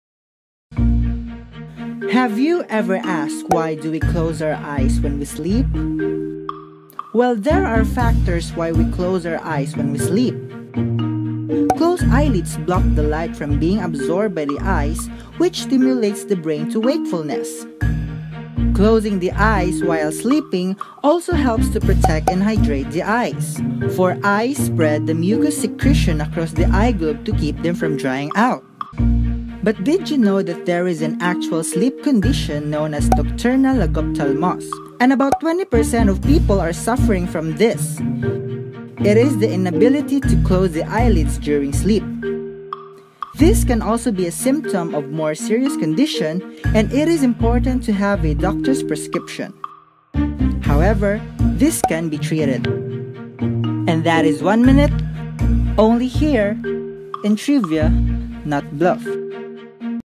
Why do we close our eyes when we sleep?
There are a number of reasons why we close our eyes to sleep. Closed eyelids block light from being absorbed by the eyes, which stimulates the brain to wakefulness. Remember, light is taken in through specialized cells (known as ganglion cells) in the retina.